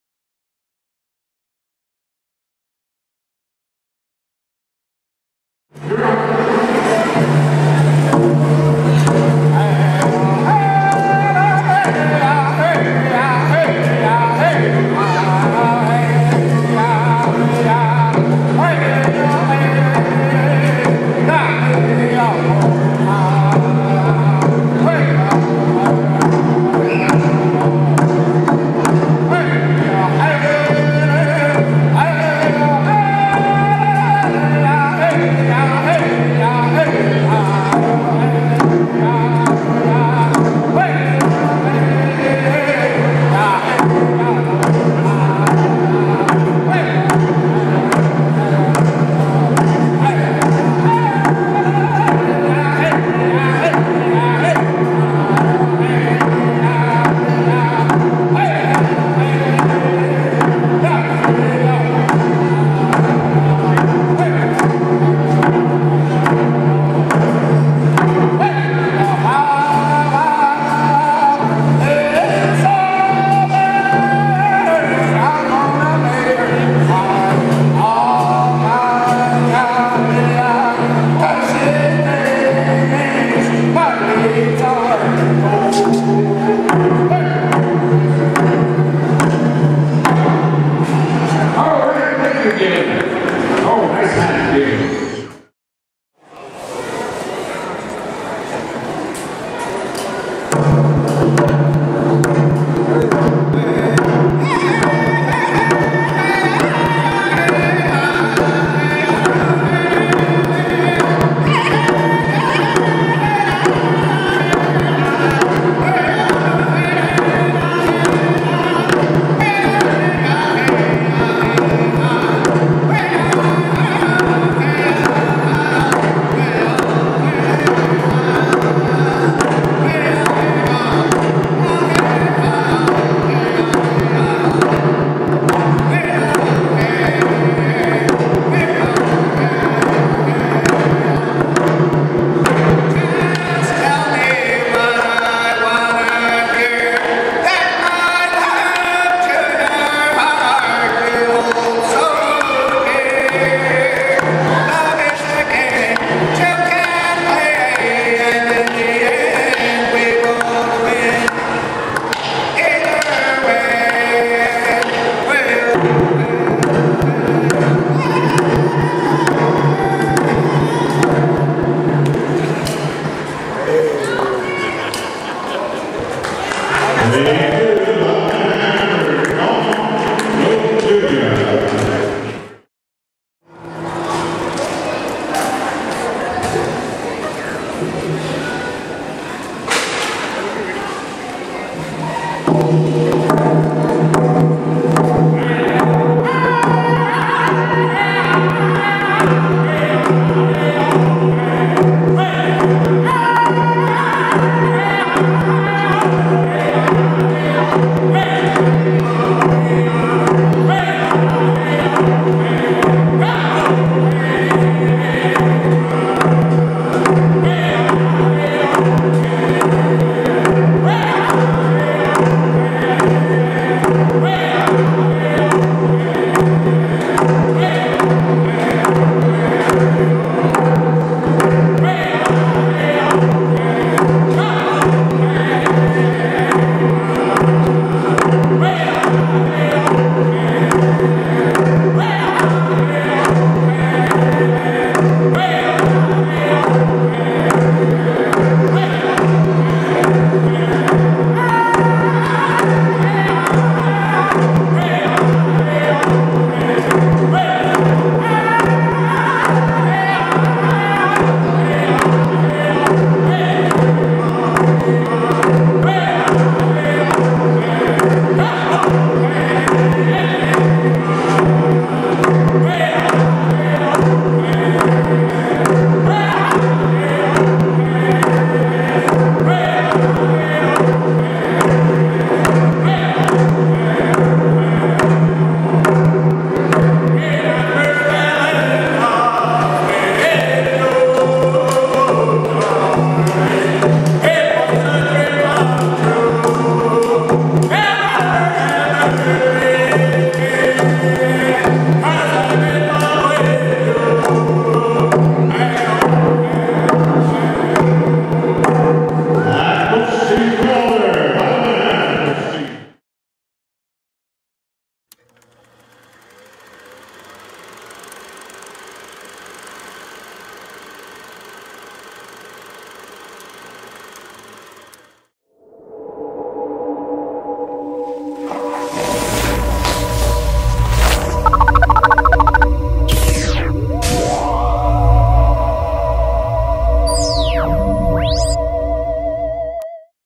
Siksika Pow Wow Hand Drummers - Group 2
Galen RidesTheGreyHorse, Len Cutter, & Steve Crawler play during the 2015 Siksika Pow Wow in Gleichen, Alberta Canada
Video by Walter B. Shepherd
TYJ